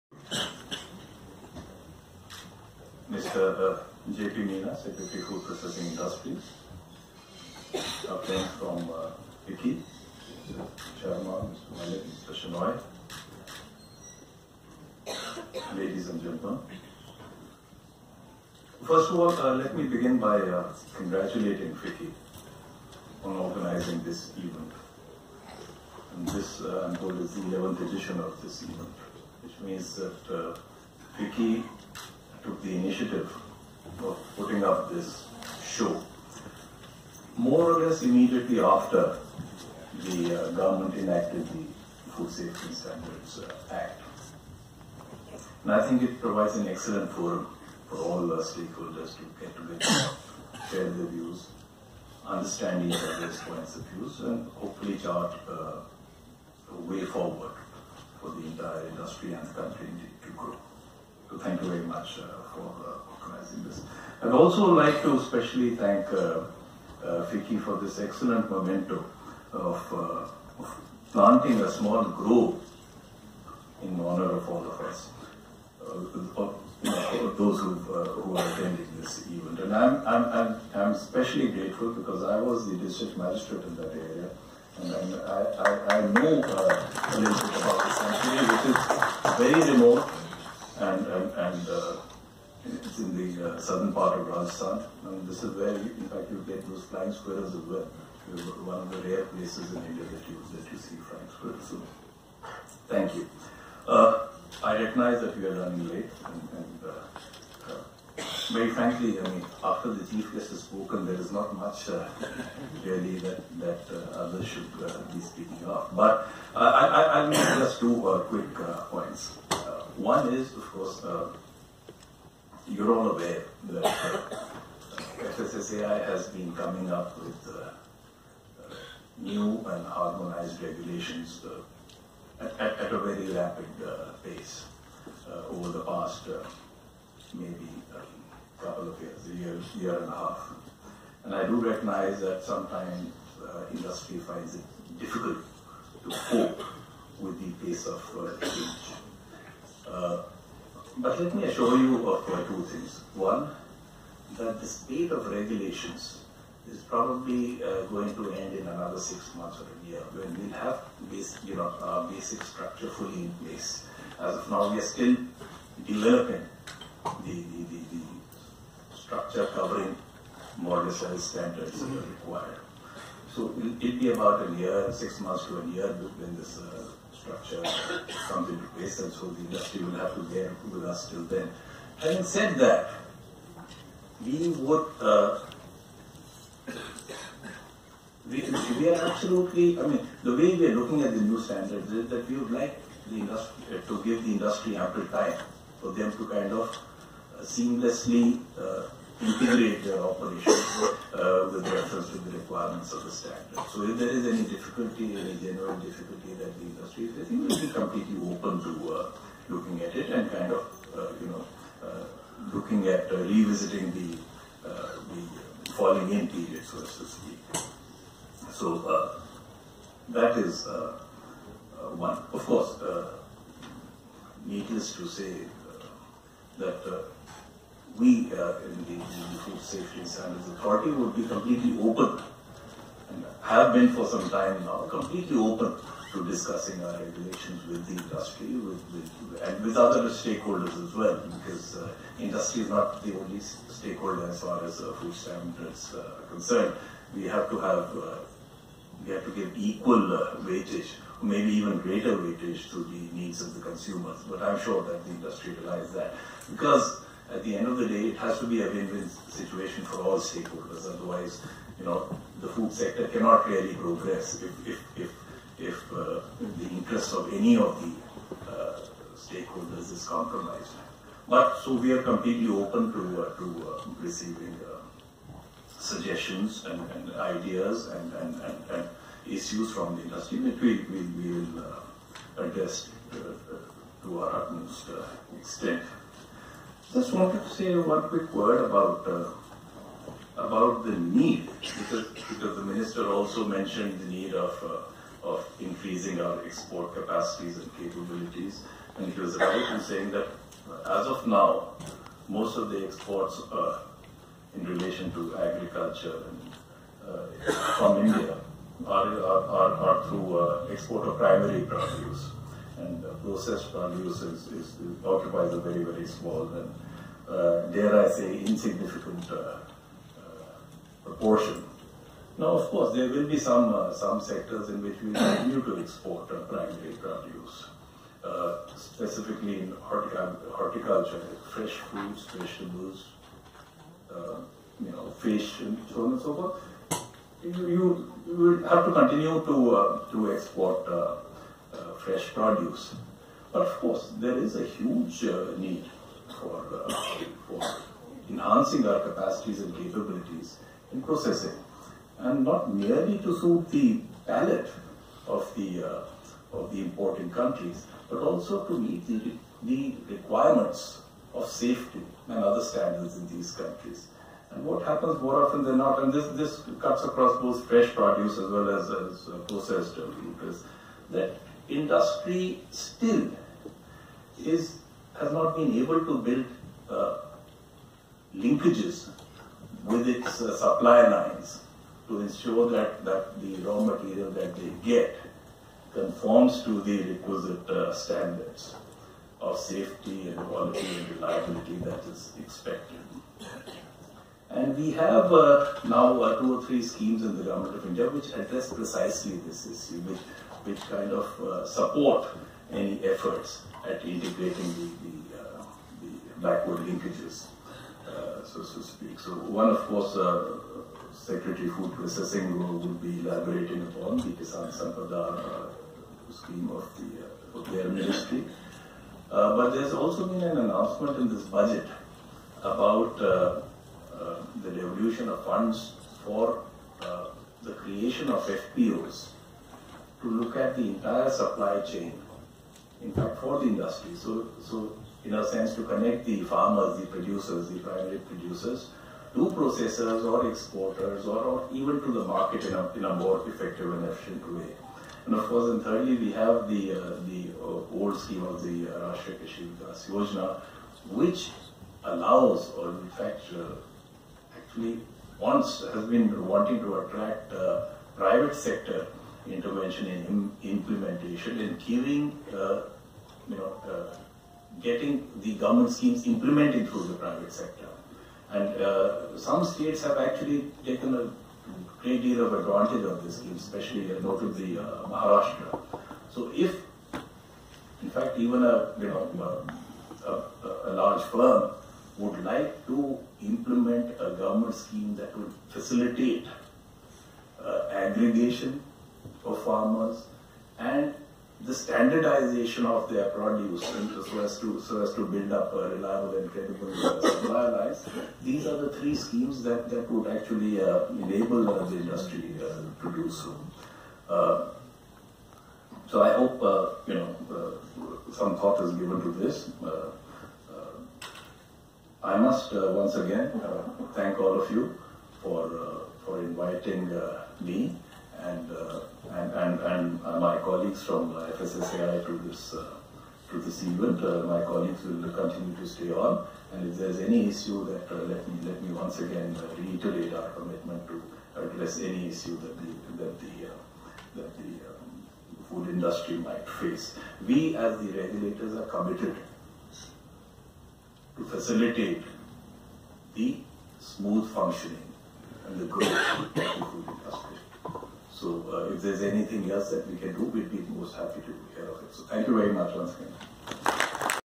Ashish Bahuguna, Chairperson, FSSAI at FOODWORLD India 2018
Ashish Bahuguna, Chairperson, FSSAI delivering special address during the inaugural session of FOODWORLD India 2018.